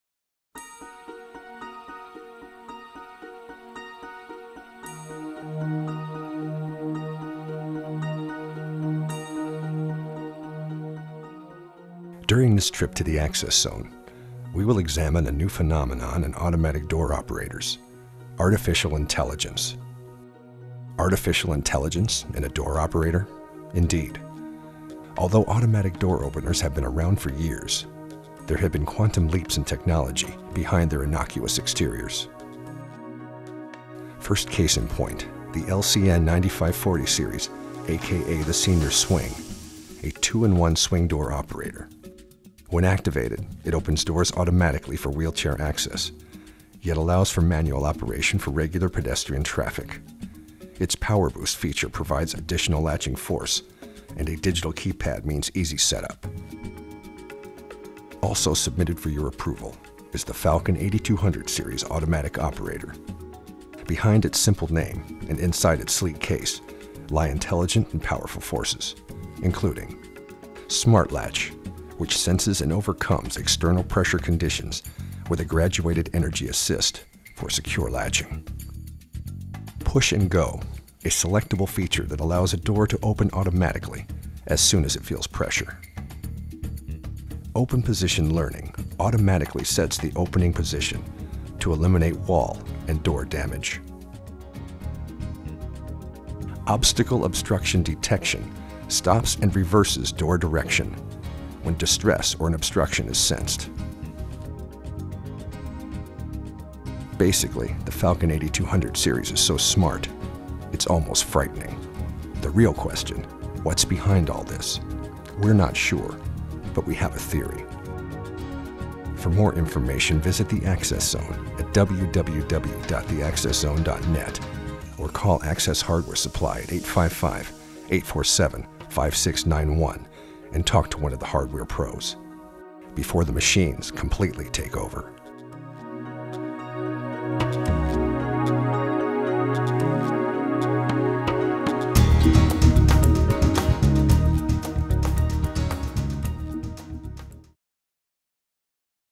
Smart Door Openers: LCN's Senior Swing & Falcon's 8200
Learn exactly what features make LCN's Senior Swing & Falcon's 8200 Series Door Openers "Smart," and how they benefit building owners and end users.